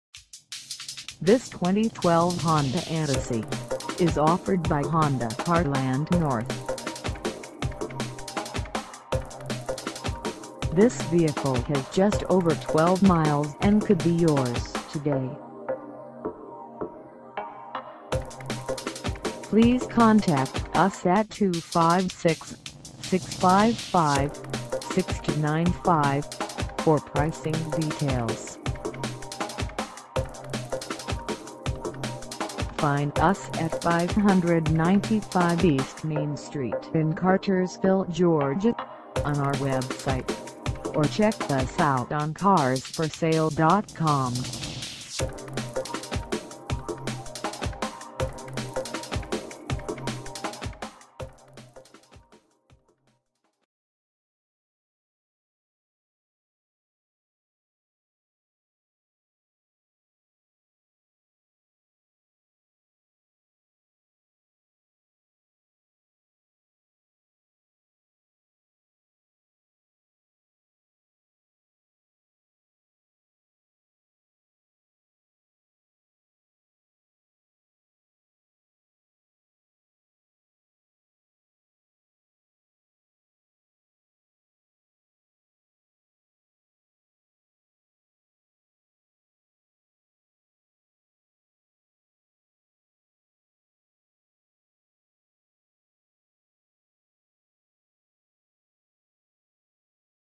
2012 Honda Odyssey EX-L - for sale in Cartersville, GA 30121
595 E MAIN ST

Come test dirve this 2012 Honda Odyssey EX-L for sale in Cartersville, GA.